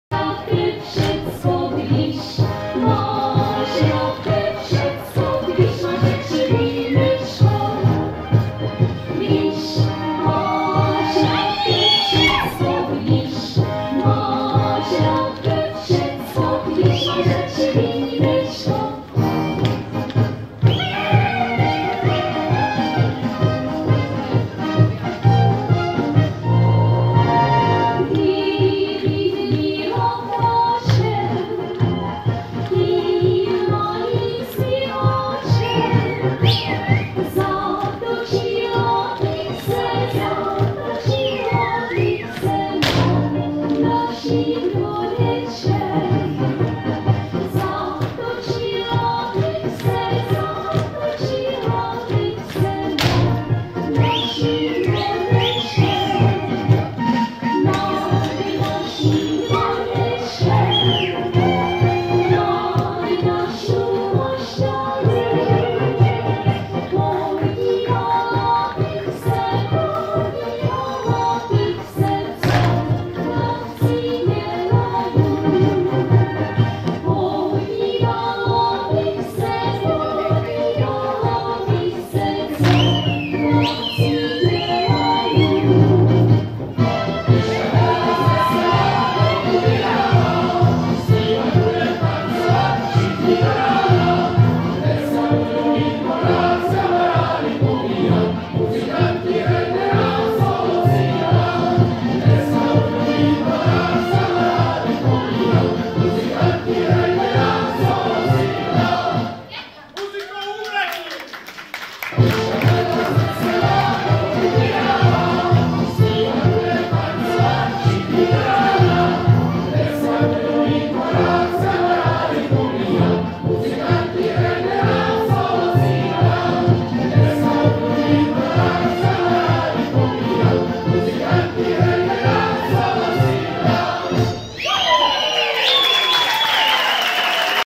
Czech Holiday Faire 2013
United Moravian Societies perform at the Czech Holiday Faire 2013.